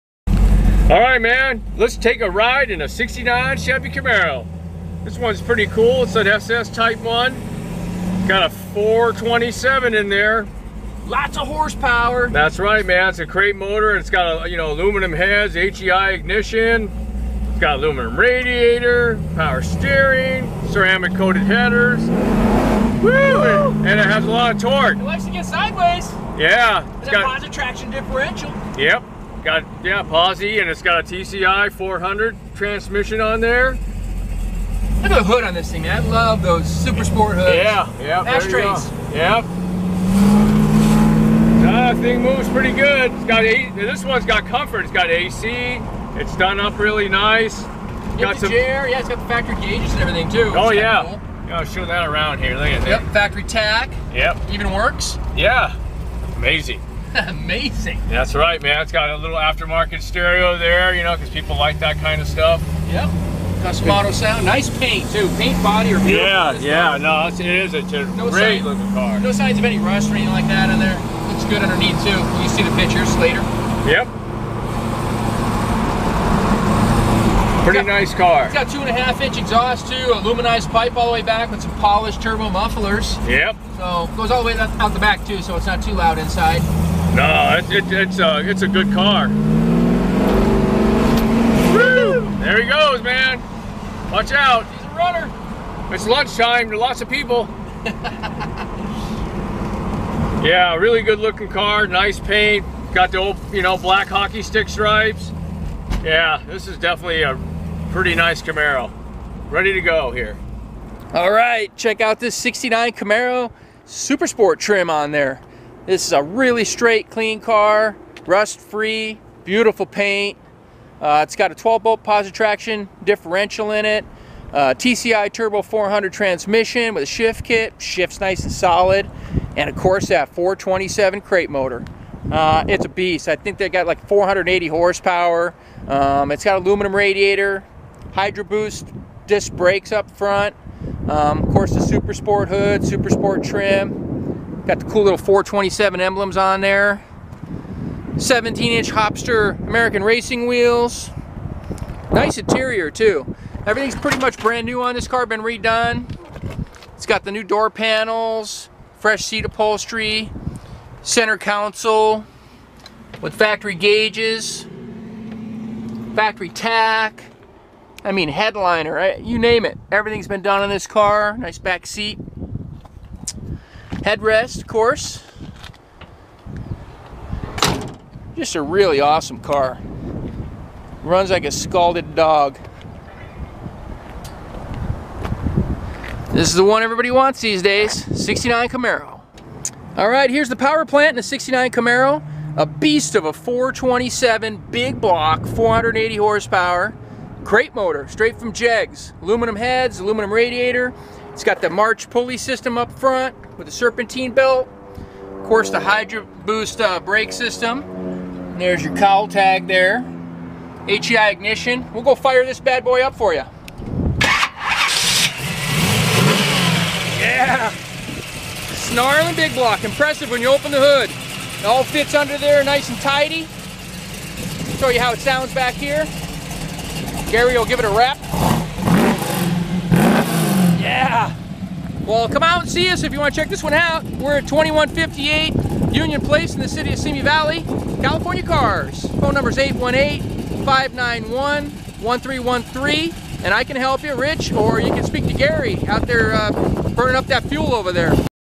1969 CHEVROLET CAMARO - SOLD
You have to like attention to own this baby! A 69 Camaro SS optioned Coupe with a ground shaking fresh 427 Chevrolet Performance crate motor with aluminum heads, HEI ignition, aluminum radiator, aluminum serpentine pulley system, electric fans, power steering, Hydroboost Disc brakes, TCI Turbo 400 transmission, Ceramic coated headers, 2.5 inch aluminized dual exhaust with polished turbo mufflers, 12 Bolt Positraction rear differential and 17 inch polished American Racing Wheels. The interior is also fresh with newly upholstered head rest bucket seats, center console with factory gauge package and floor shifter, factory tachometer, Vintage A/C, custom auto sound stereo and stunning medium Blue metallic paint with Black hockey stick stripes. This one will make you feel like a kid again!
Body: Coupe
Color: Blue
Stock ID: 200206
VIN: 124379N579717